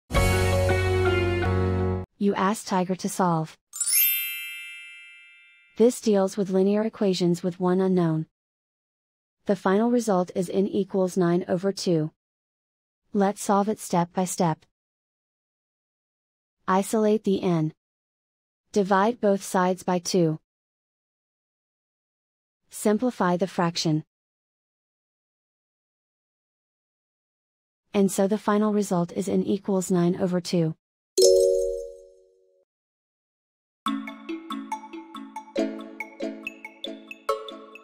Linear equation with one unknown: Solve 2n=9 step-by-step solution
Solving 2n=9 using the linear equation with one unknown solver

for a step-by-step and more tips and tricks for dealing with linear equations with one unknown.